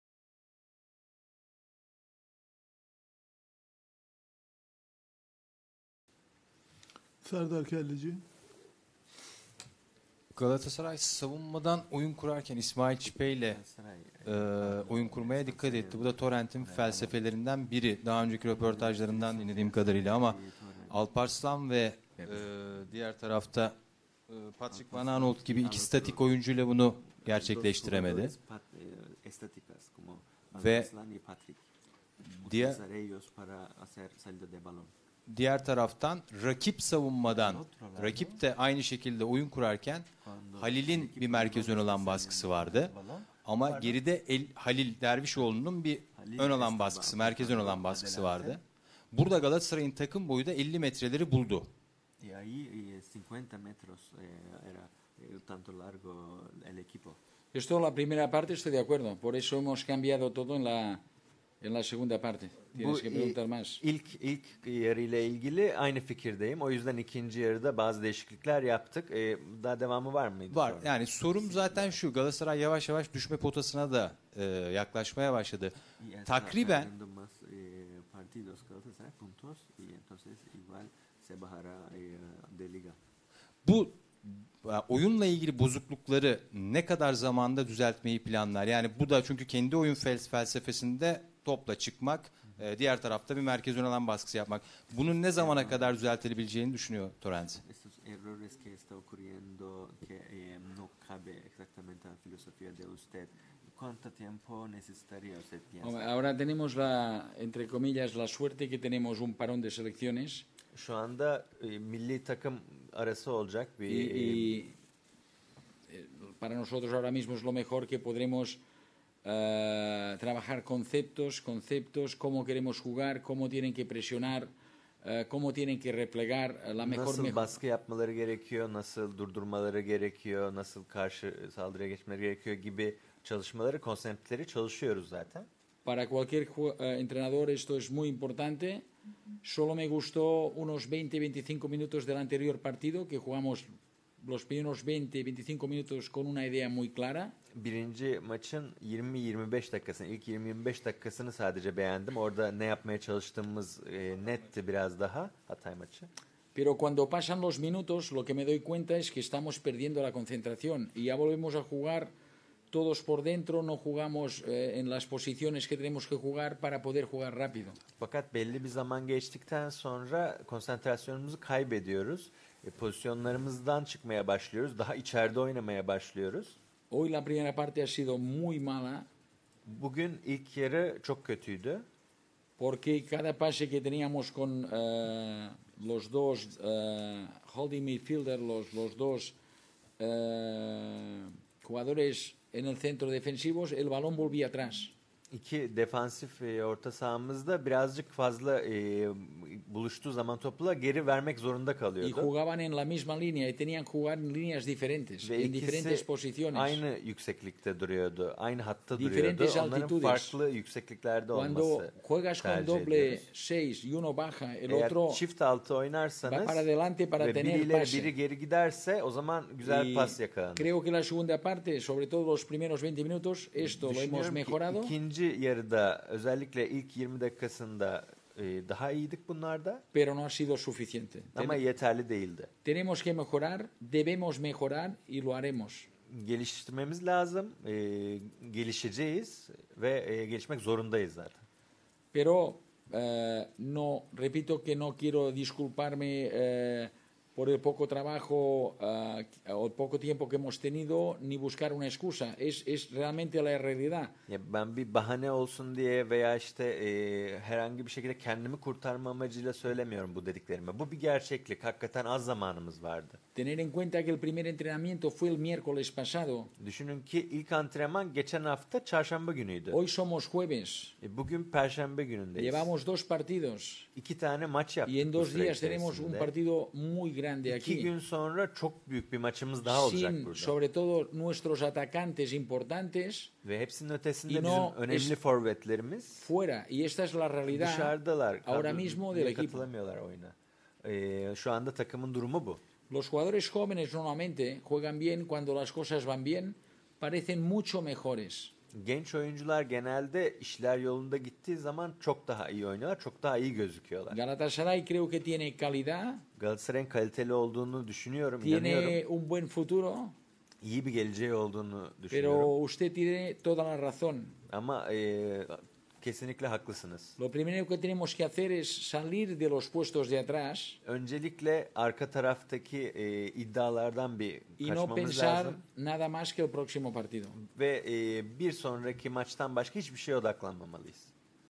Domenec Torrent, Trabzonspor maçını işaret etti: 2 gün sonra...
GÖRÜNTÜ DÖKÜMÜ :
- Galatasaray Teknik Direktörü Domenec Torrent'in basın toplantısı İSTANBUL (AA) - Galatasaray Teknik Direktörü Domenec Torrent, Spor Toto Süper Lig'in 22. haftasında oynadıkları Kasımpaşa maçının ardından basın toplantısı düzenledi. (3)